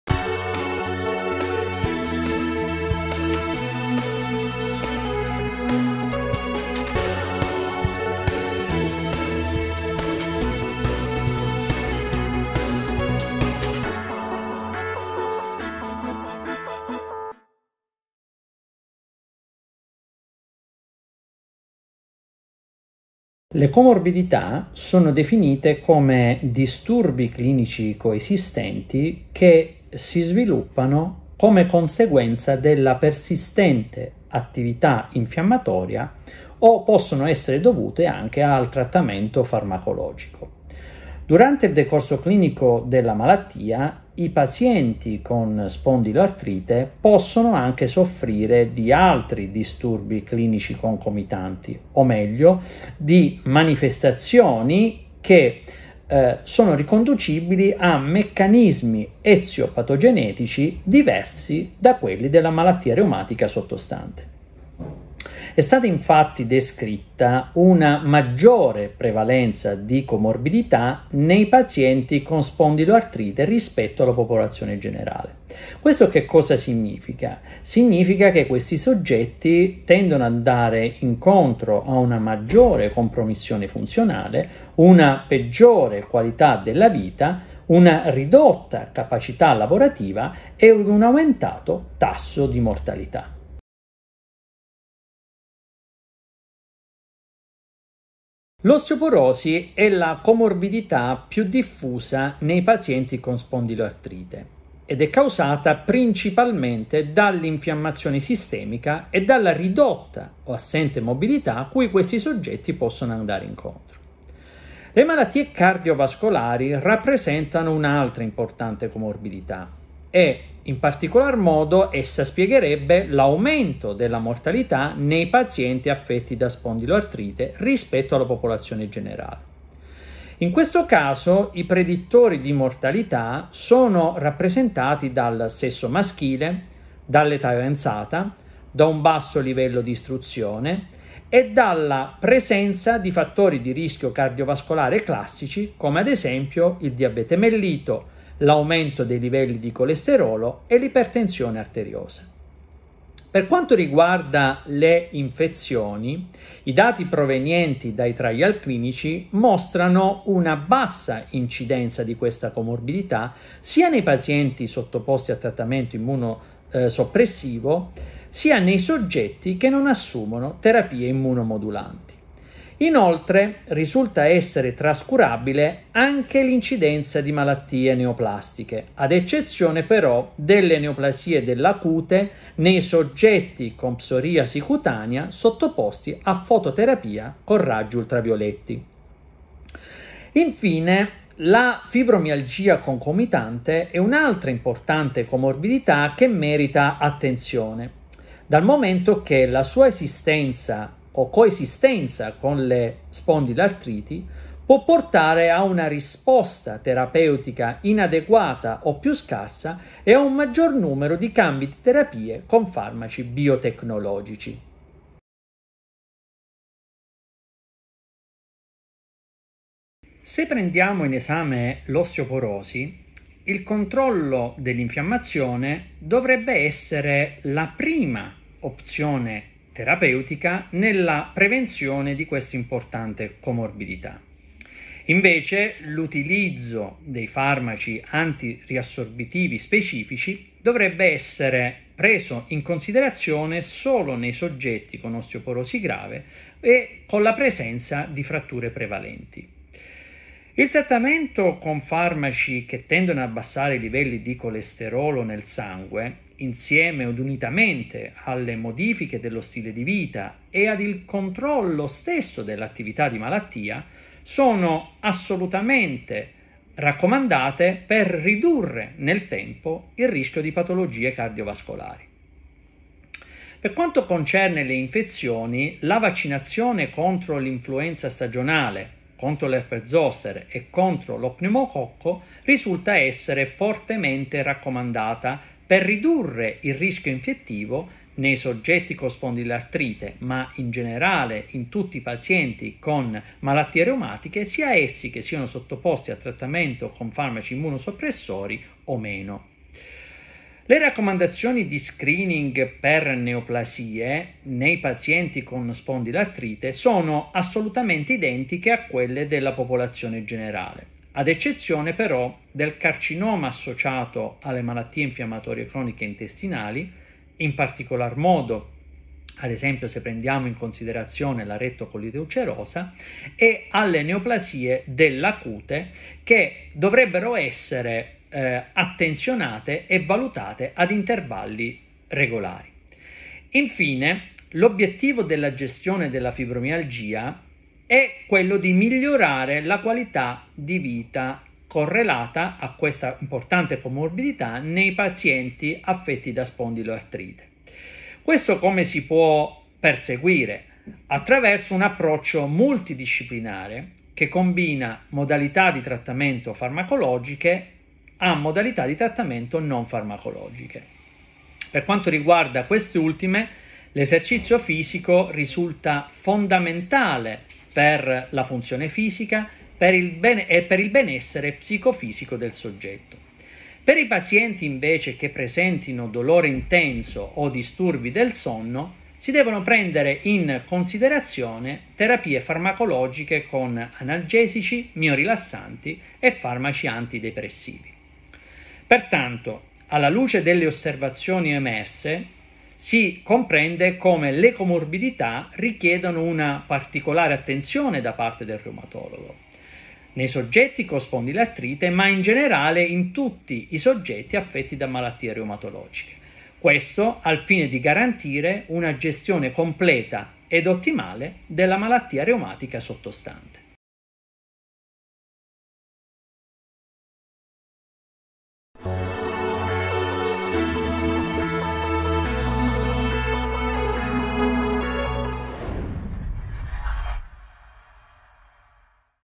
Le comorbidità nel paziente con Spondiloartrite
Durante il decorso clinico della malattia, le persone affette da spondiloartrite possono sviluppare diverse comorbidità, che incidono sensibilmente sulla qualità di vita.
Tra queste, la più diffusa è l’osteoporosi.

Guarda la clip con Giuseppe Lopalco, Professore Associato in Reumatologia presso l’Ospedale Policlinico | Università di Bari.